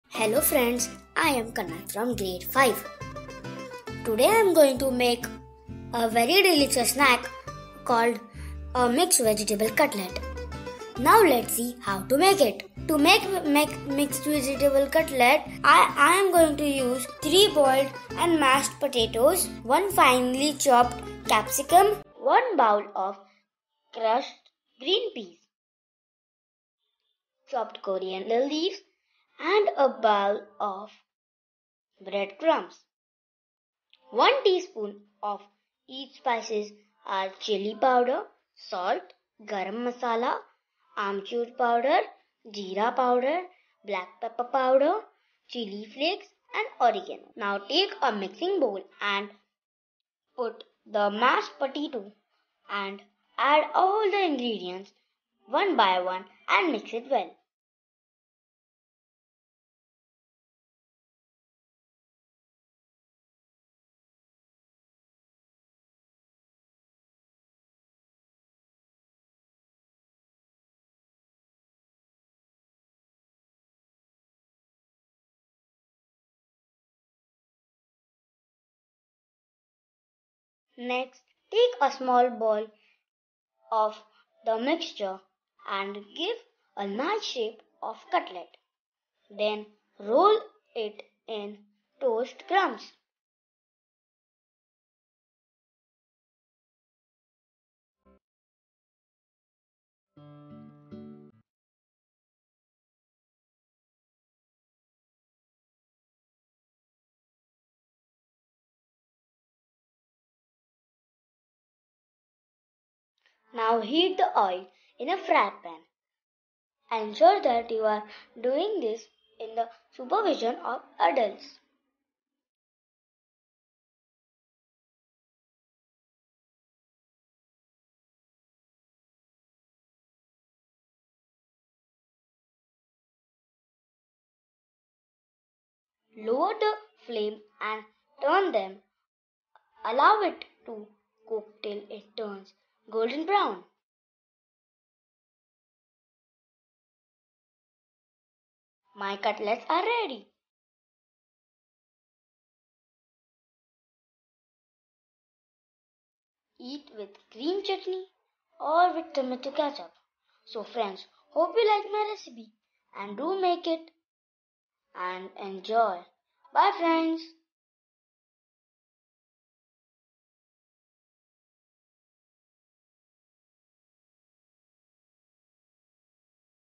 Vegetable Cutlets - Crispy, Crunchy & Delicious Veg Cutlets recipe by little chef
Today's recipe is MIX VEGETABLE CUTLETS, A quick snack recipe that can be made at home with very few ingredients. Ingredients used like vegetables like 3-4 Boiled Potatoes, Capsicum, Green Peas, & Carrots, etc. To make spicier you may add red chilli and Peeper as well. Serve these cutlets warm green chutney and tomato sauce or mint coriander chutney. I hope you will enjoy watching this video. Thanks! Ingredients: 3 Boiled and mashed potatoes 1 Finely chopped capsicum 1 Grated carrot 1 Bowl of crushed green peas Few Chopped coriander leaves 1 A bowl of bread crumbs 1 Teaspoon chilli powder 1 Teaspoon salt 1 Teaspoon garam masala 1 Teaspoon amchur powder 1 Teaspoon jeera powder 1 Teaspoon black pepper powder 1 Teaspoon chilli flakes 1 Teaspoon oregano 1 A bowl of toast powder Leave me comments down below because I love reading them! Also, I would love suggestions from you.